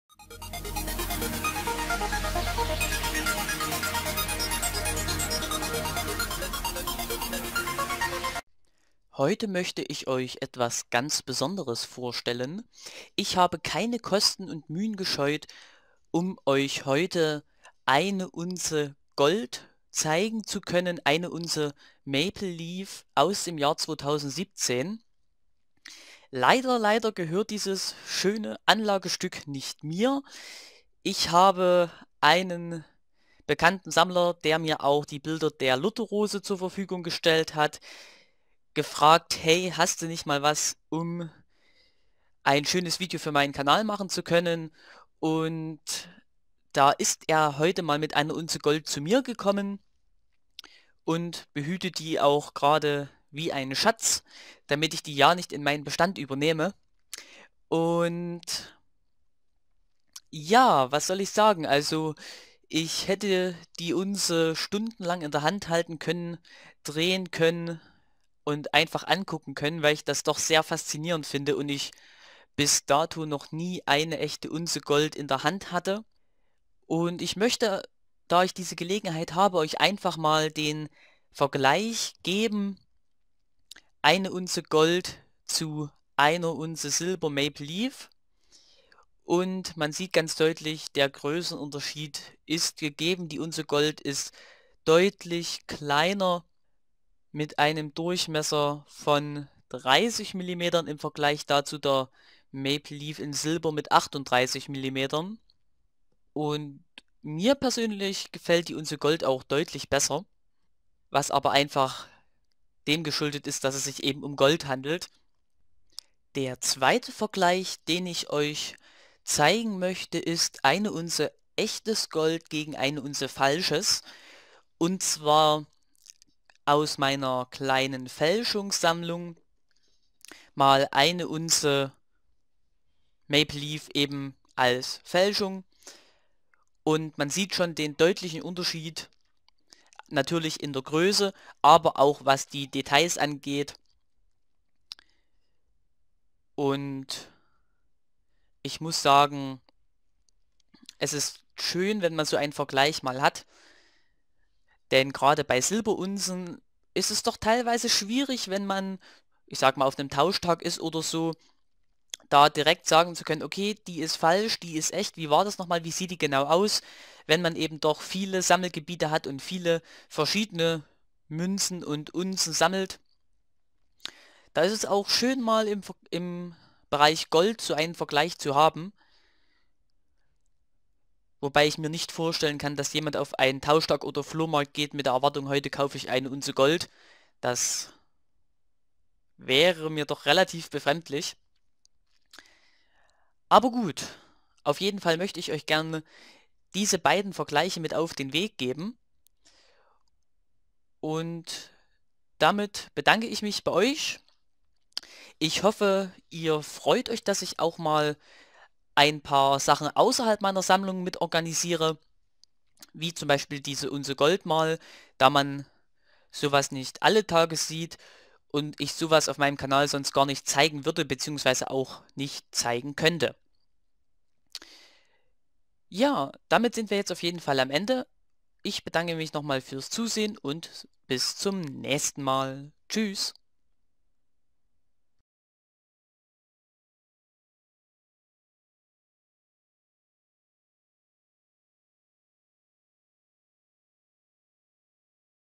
Maple Leaf in Gold - 1 Unze Feingold
Ich freue mich besonders euch heute eine Unze Feingold auf meinem Kanal vorstellen zu können. Leider gehört dieses gute Stück nicht mir, aber ich habe keine Kosten und Mühen gescheut, damit mich ein befreundeter Sammler besucht.

Mich hat es faszinierd eine Unze Gold in den Händen zu haben und sich dabei Zeit lassen zu können. In dem Video gehe ich einerseits auf den Vergleich Silber Maple Leaf und Gold Maple Leaf ein, aber auch auf eine Fälschung.